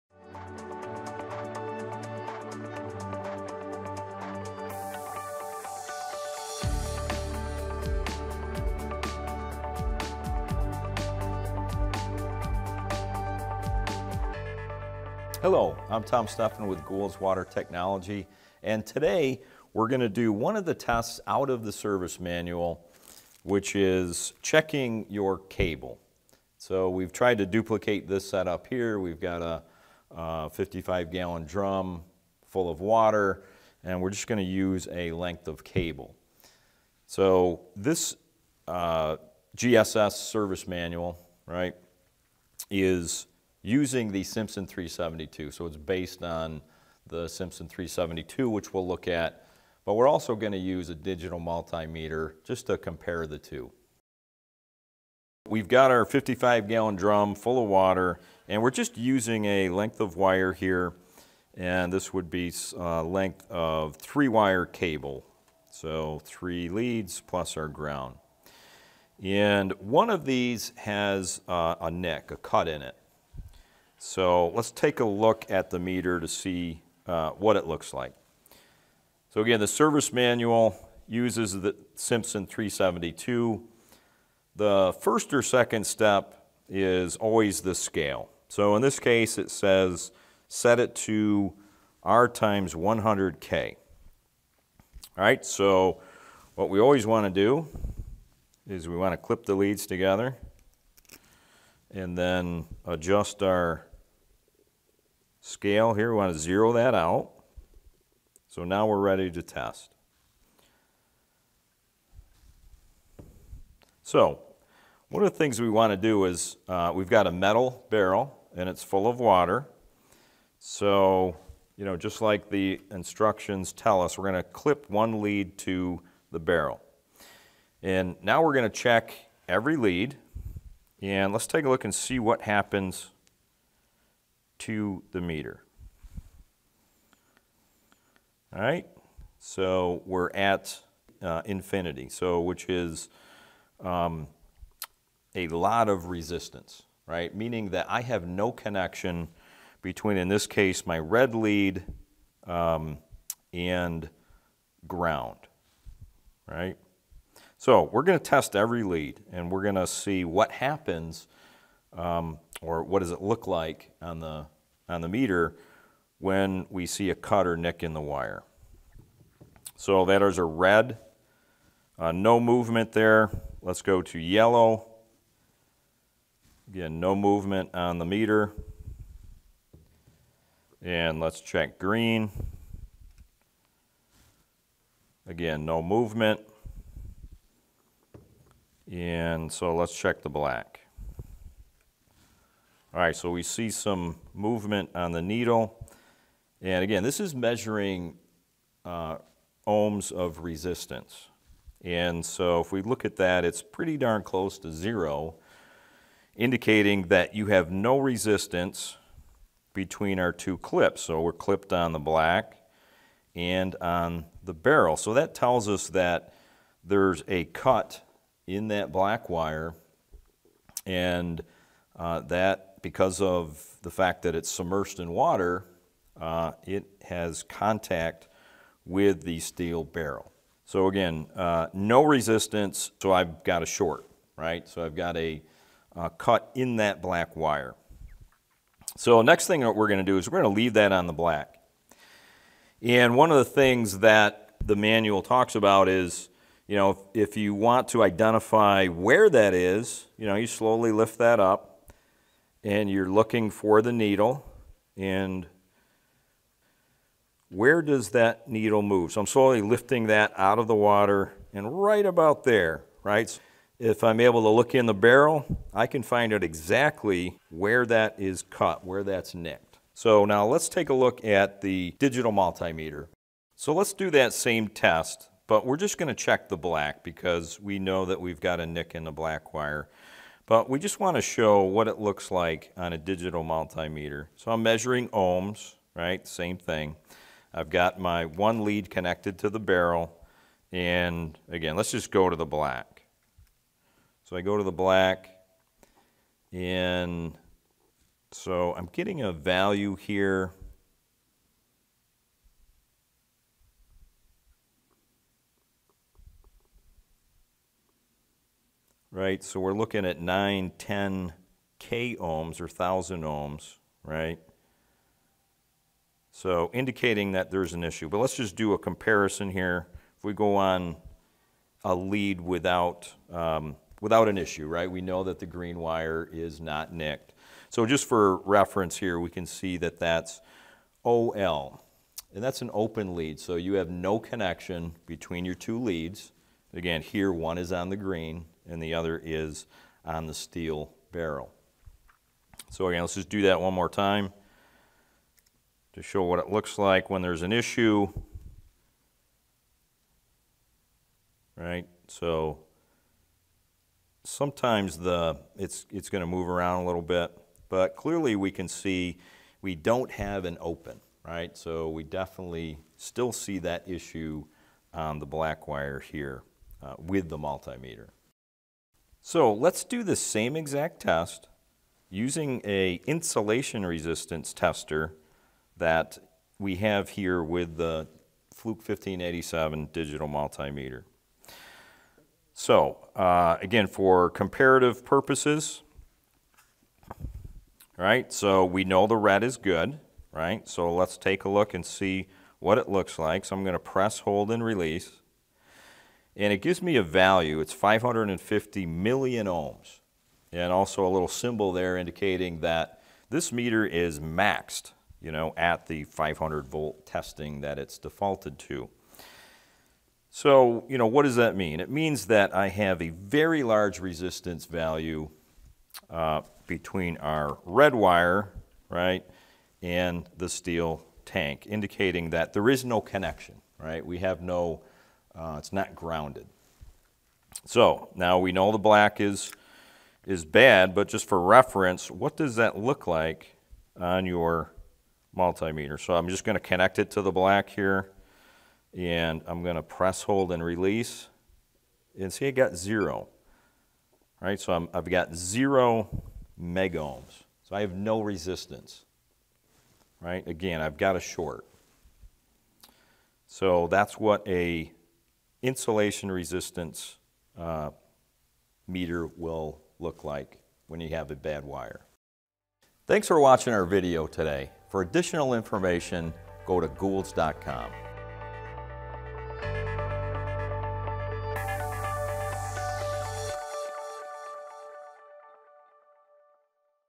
Submersible Pump Cable Checkout Procedure
Follow along as Tom Stephan, Training Manager, walks through the proper checkout procedure for a Goulds Water Technology Submersible Pump Cable.